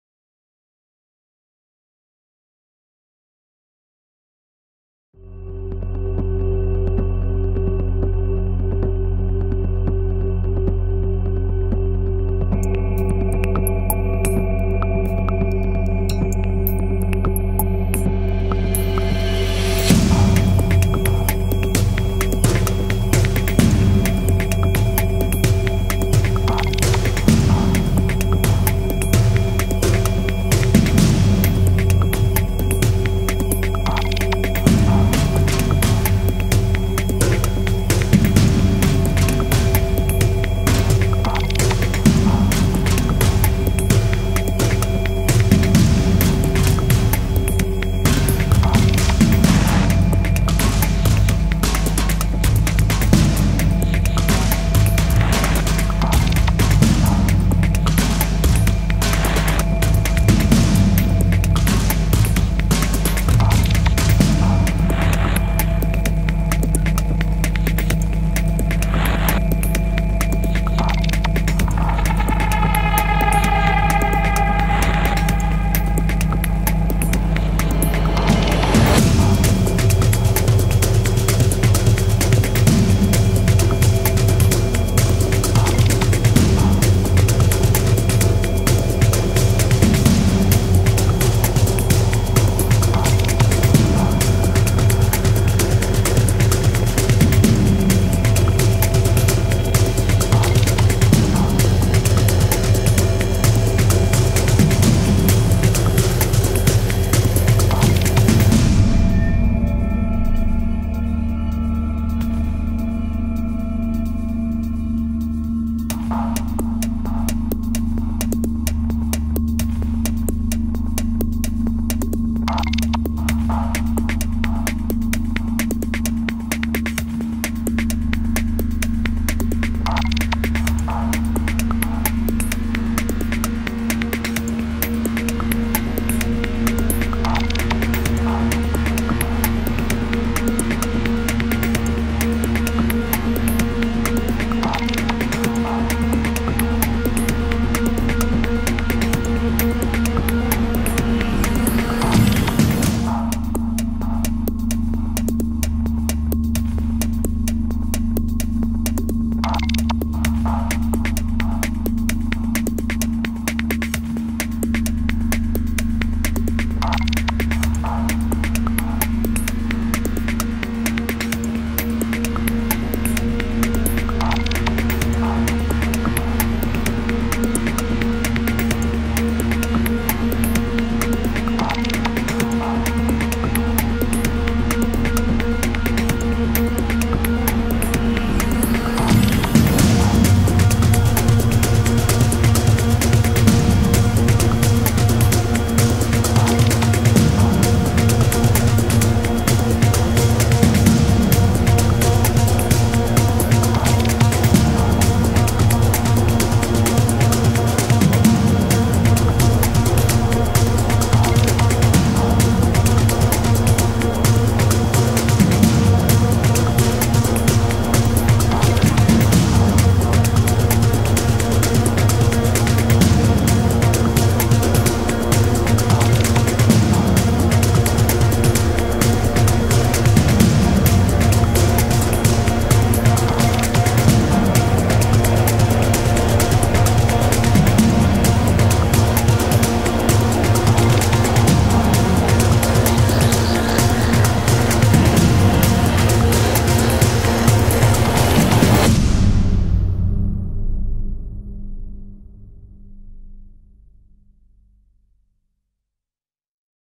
System LAMGEA How It's Made
The LAMGEA system involves laying powder directly on the belt without special trolleys (single and multi-load) making the pressing process even easier.